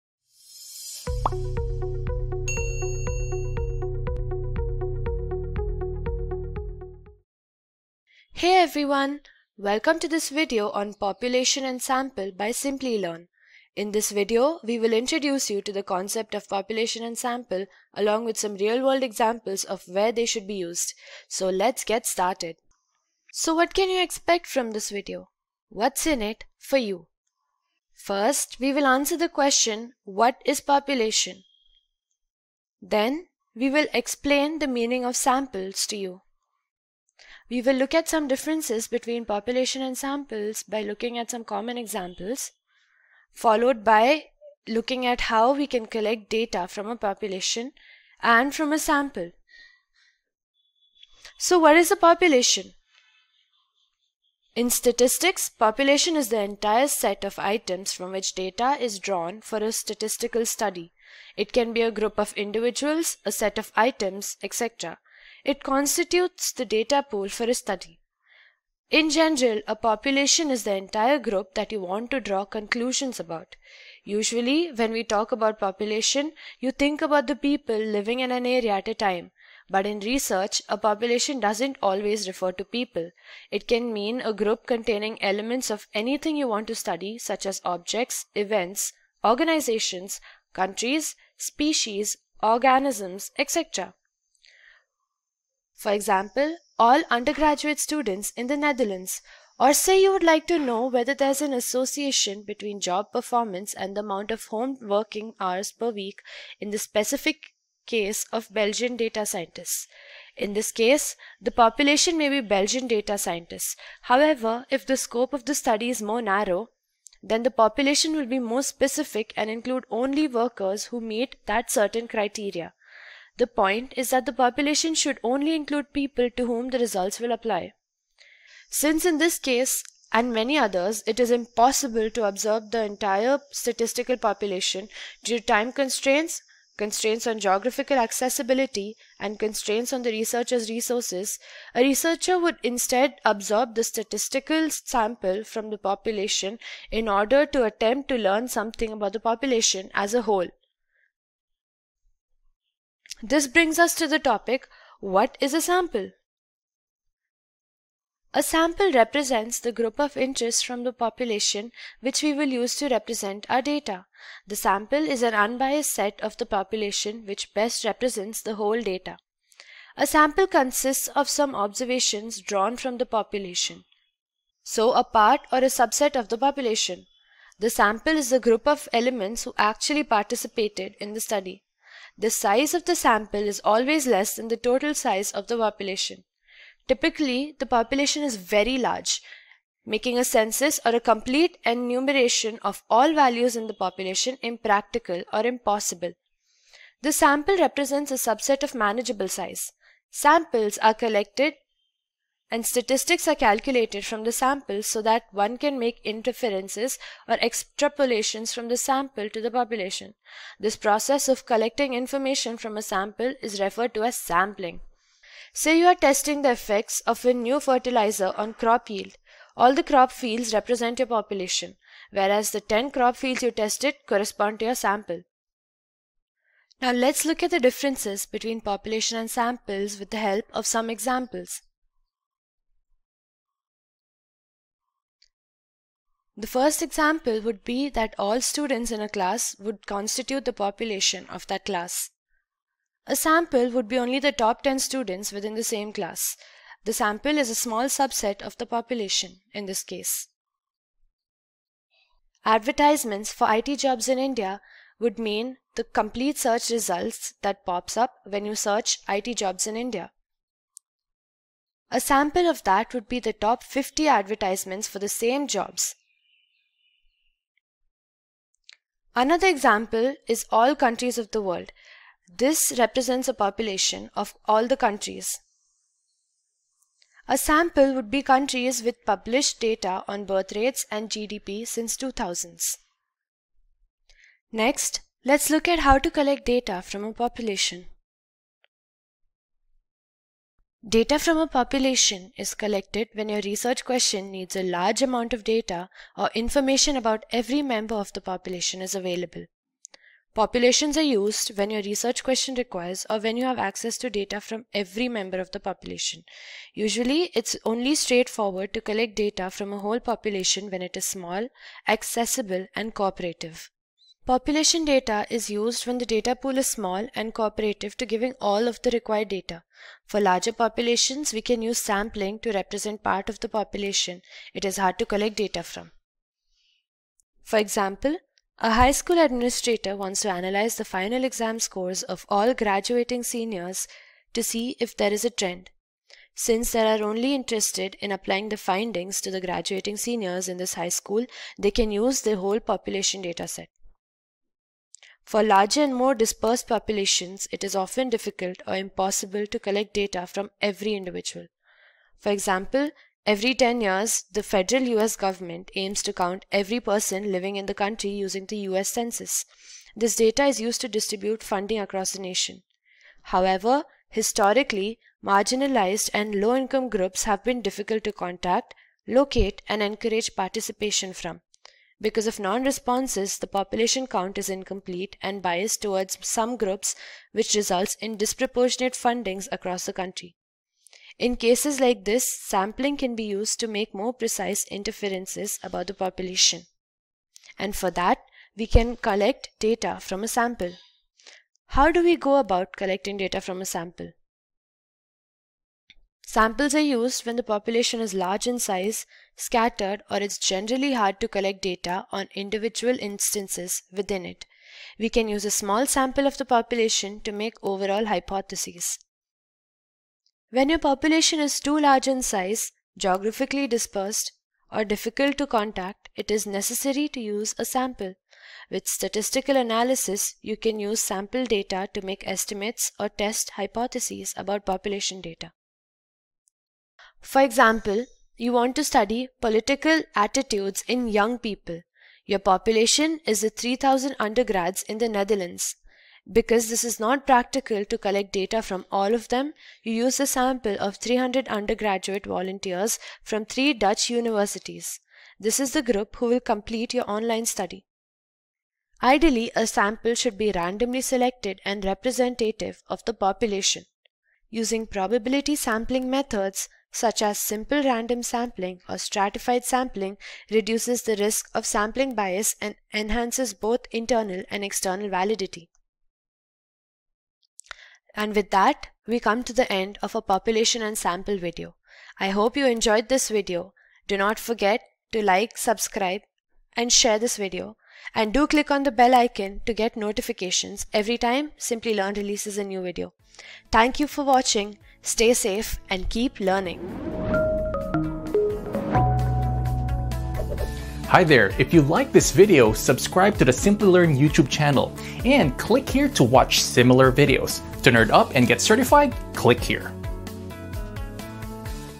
Population And Sample In Statistics Example | Population vs Sample In Statistics | Simplilearn
🔥Professional Certificate in Data Science and Generative AI, delivered by Simplilearn in collaboration with Purdue University

In this video, Population vs Sample in Statistics, we will first answer the question what is population and sample. We will then take a look at the difference between population and sample along with Population And Sample In Statistics Examples. Finally, we will see how to collect data from population and sample.

00:00 What is Population
02:38 What is a Sample
03:48 Difference betweer Population and Sample
04:48 How to collect data from a population
06:38 How to collect data from a sample

✅ Skills Covered
- Data Analytics
- Supervised Learning
- Unsupervised Learning